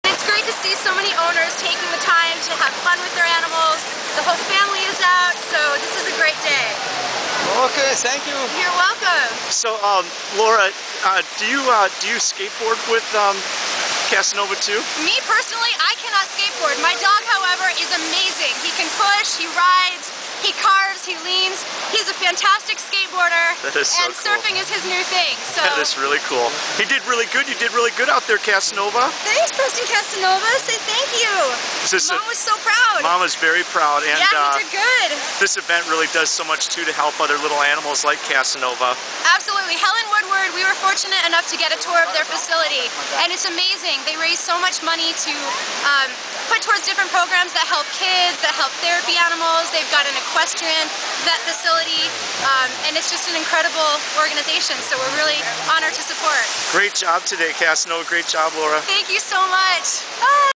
Preston Casanova and Mom Laura 1 - 2009 Dog Surfing Helen Woodward
- Dog Surfing Photos and Video Coverage

Check out a quick clip from the 2009 Helen Woodward Dog Surfing Event at Dog Beach in Del Mar featuring Preston fropm "Prestons Planet" along with Mom and owner Laura. These little dogs have a lot of courage and surf to raise money for homeless pets and animals. Preston was featured in the CBS reality show "Greatest American Dogs"

For more information on the Helen Woodward Animal Shelter and their programs